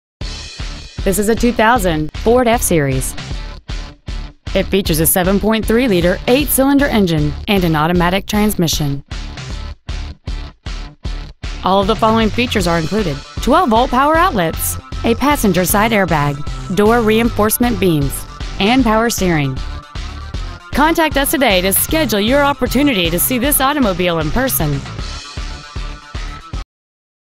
Used 2000 FORD F-350 Chandler AZ
Year : 2000
 Make : FORD
 Model : F-350
 Engine : 7.3L V8 16V DDI OHV TURBO DIESEL
 Trans . : AUTOMATIC
 Exterior : TAN
 Miles : 84,360
 Interior : BEIGE
 Stock : FC2059AA